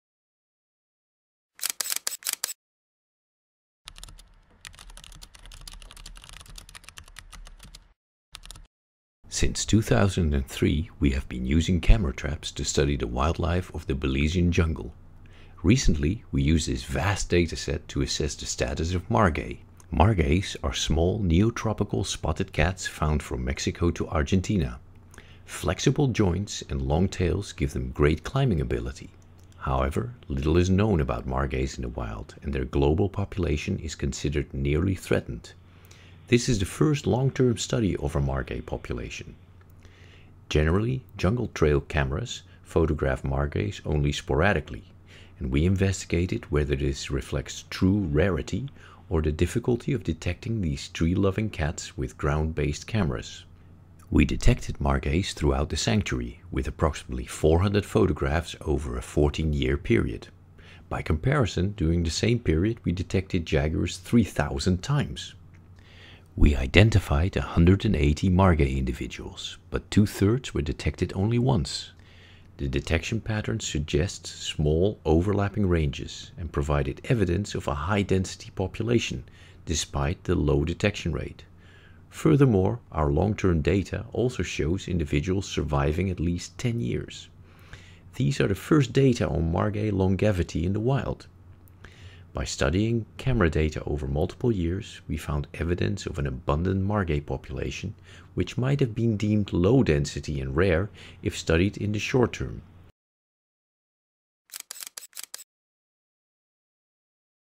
60 Second Science: The Mysterious Margays of Belize
In this 60SecondScience video, Dr. Bart Harmsen talks about his team's long-term study on margays in Belize. Their work, collected over a 14 year period, helps them better understand the population and behavior of these smallcats, including presenting some of the first-ever data on wild margay longevity.